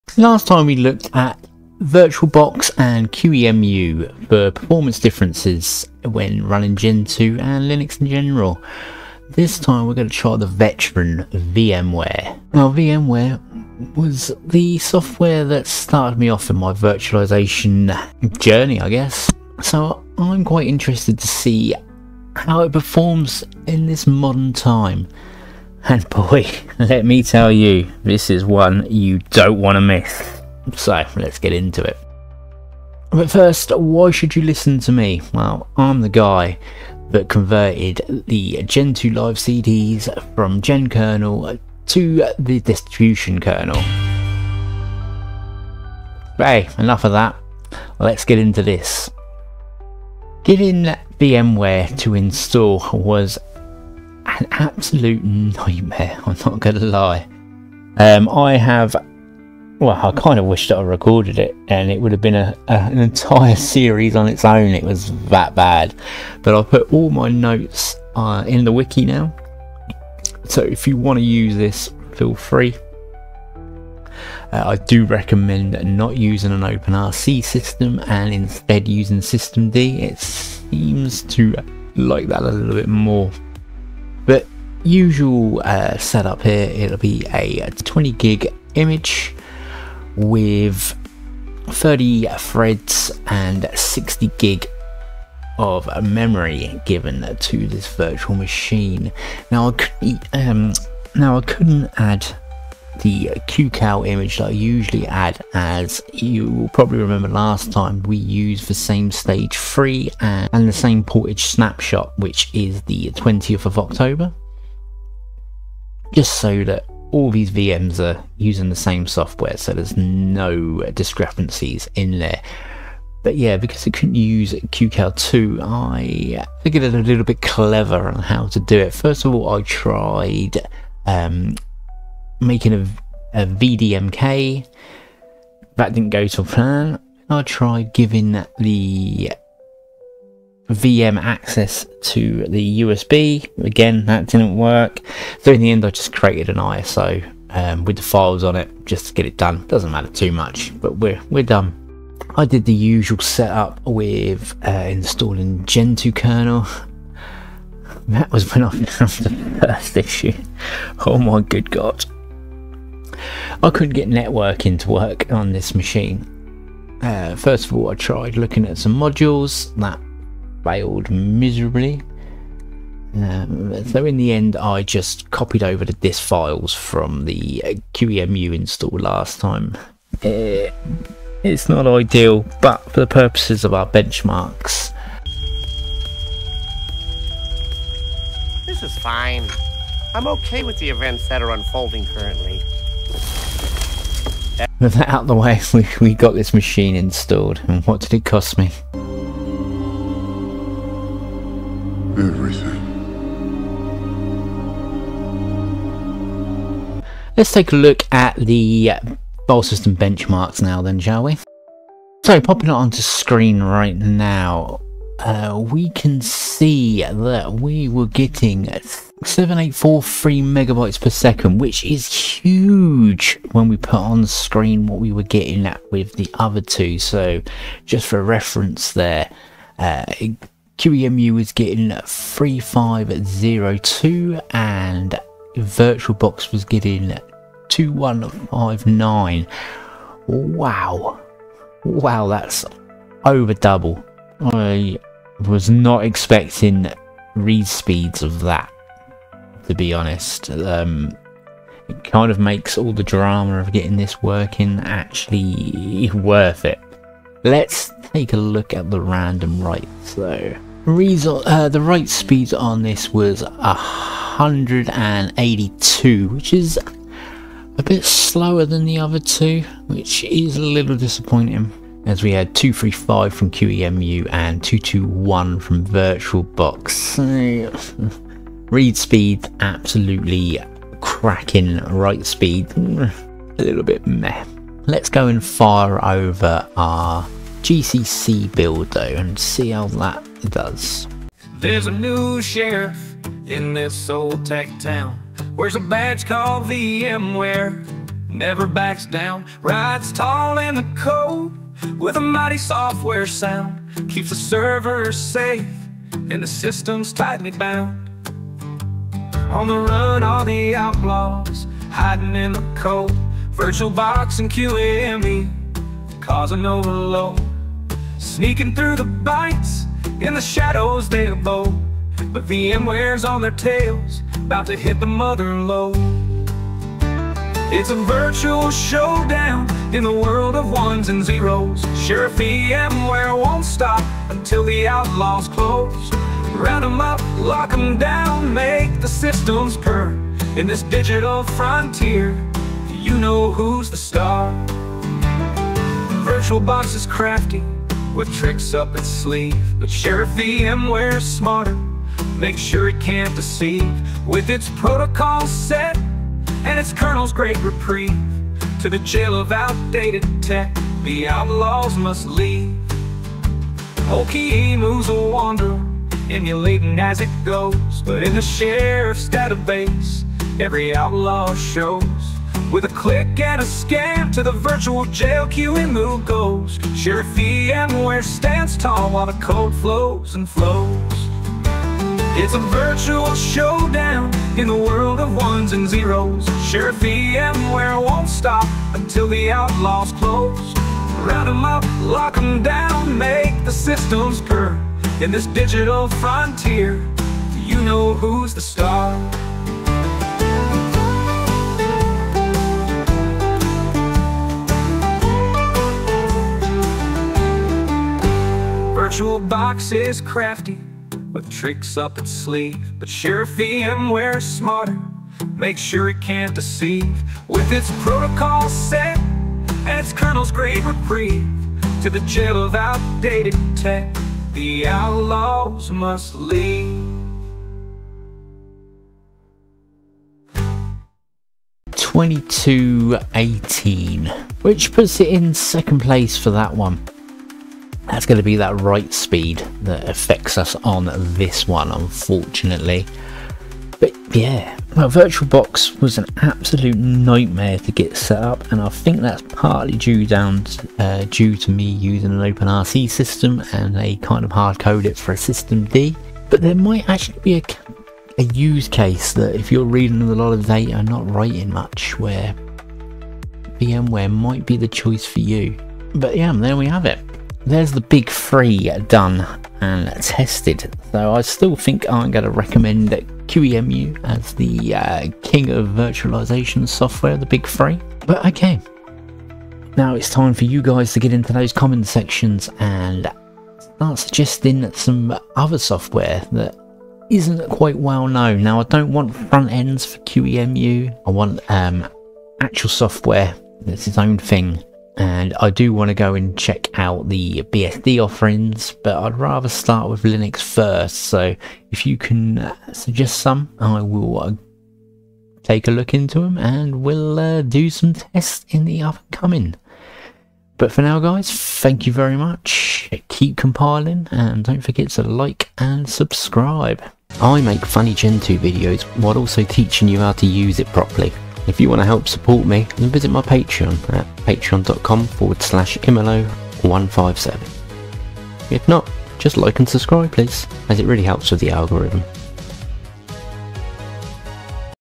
VMware V VirtualBox V QEMU - Performance Compared
I couldn't find a video a video that compares VM software so here is that video in full HD glory!

Thumbnail Image - Created by Sebastian Pipping and Ethan Dunham